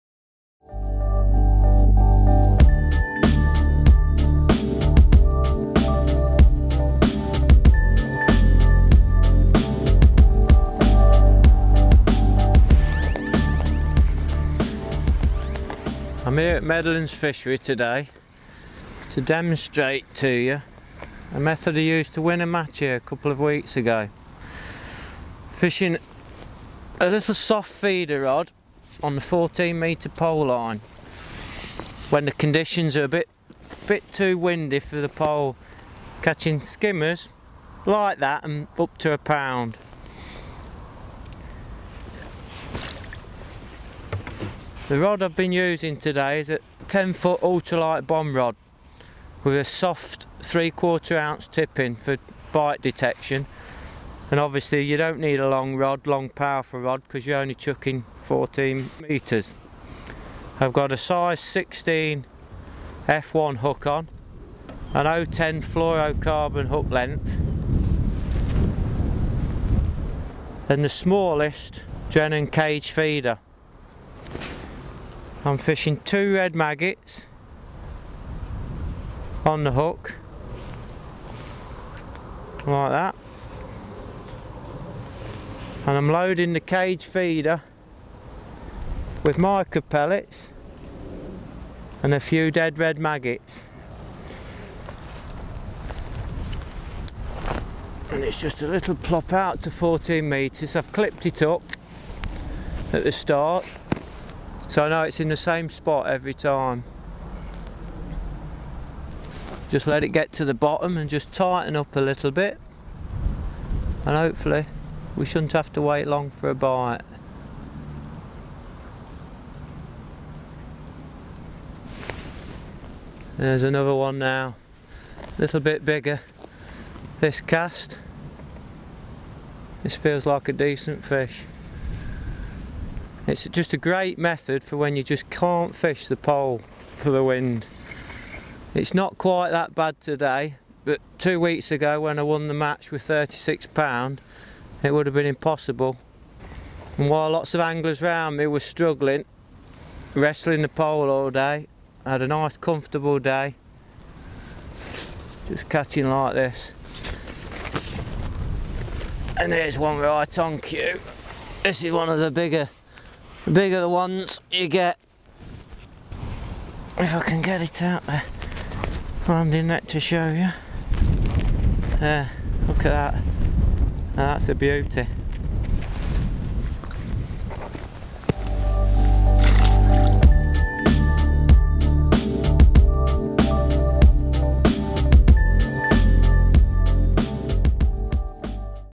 Winning Tactics: Short-Range Feeder
Steve Hemingray visits Meadowlands Fishery to show you a really simple and effective way to catch skimmers at close range with light feeder tactics.